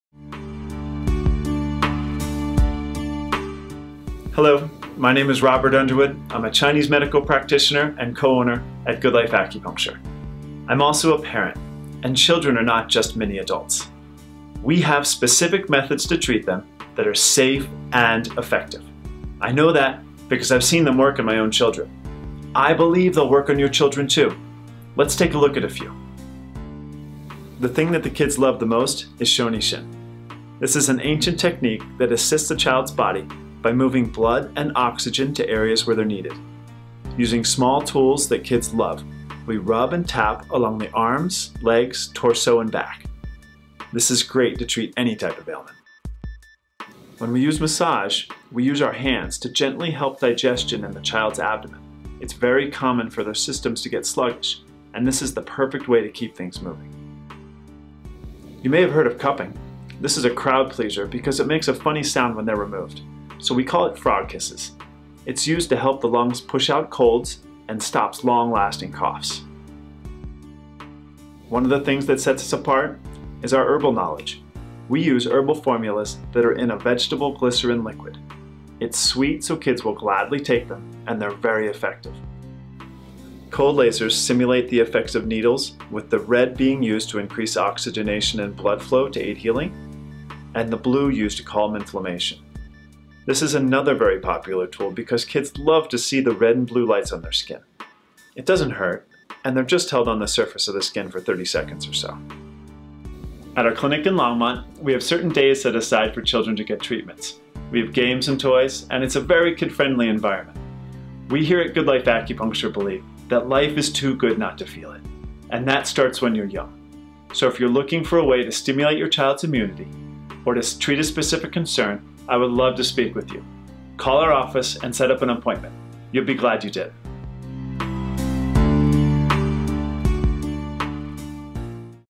Good Life Acupuncture & Wellness Center
Introduction Video for acupuncture for kids at Good Life Acupuncture & Wellness Center.